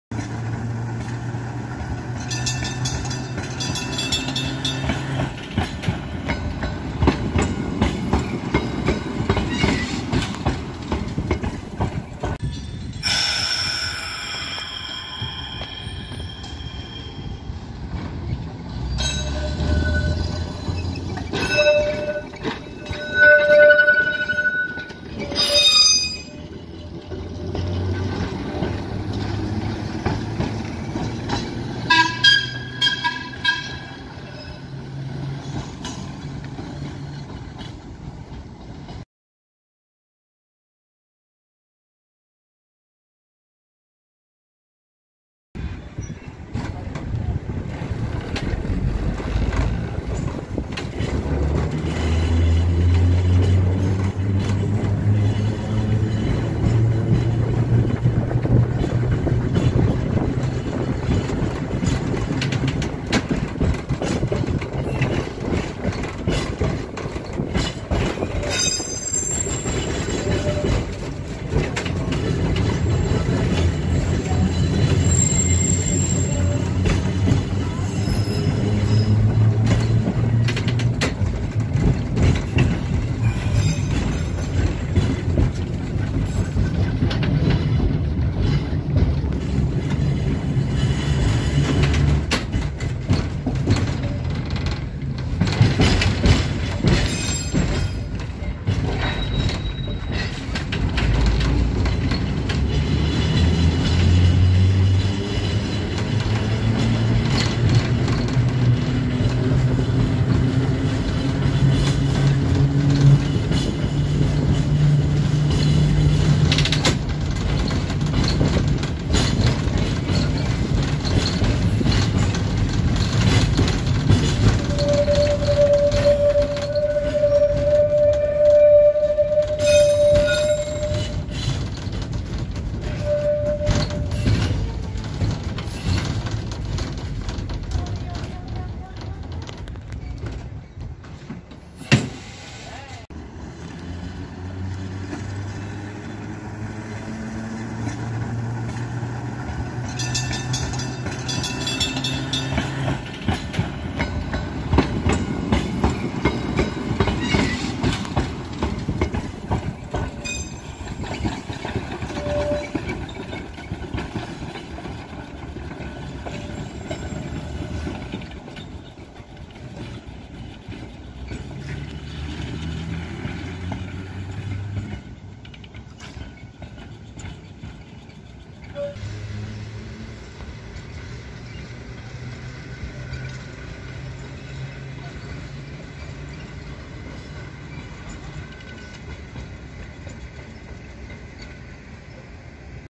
The Return of The History Park Trolleys!
On September 25th, 2021, San Jose Railroads #124 led the first streetcar run in the History Park for the first time in awhile.

Of course, I had to be there (on the day after!).